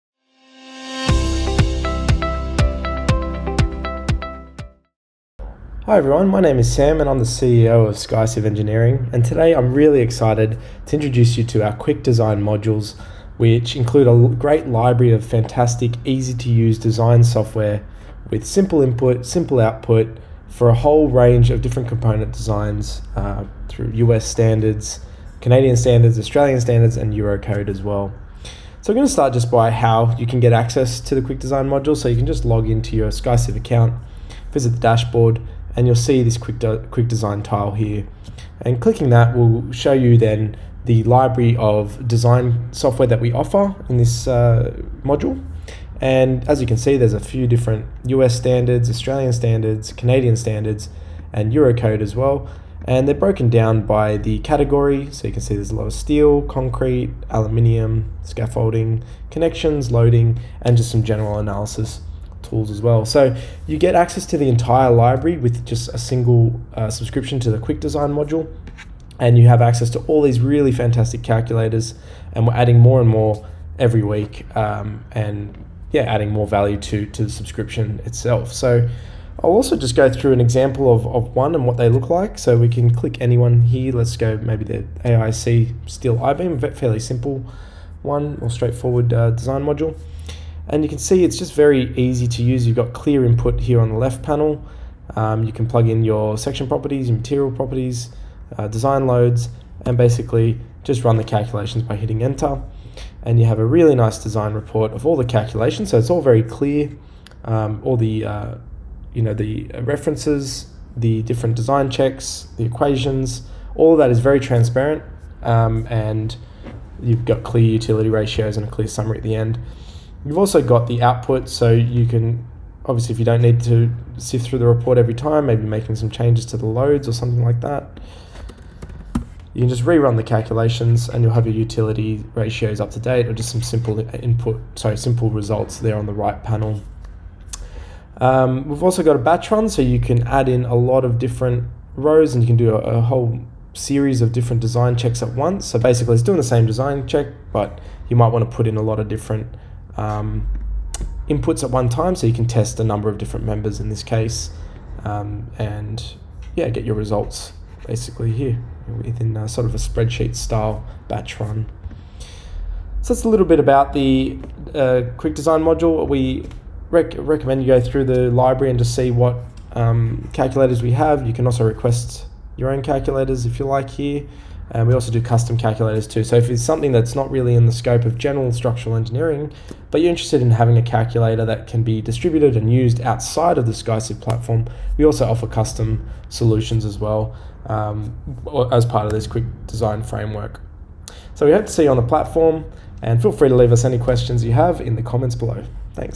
Introducing SkyCiv Quick Design
We are thrilled to introduce the SkyCiv Quick Design, your ultimate engineering design toolbox.

Quick Design is a new way to quickly complete design checks from standards around the world with an intuitive, easy-to-use interface. It features a library of over 50 calculators based on US, Australian, Canadian, and European standards. Additionally, the Quick Design module offers a core set of basic analysis tools that are valuable for every structural engineer.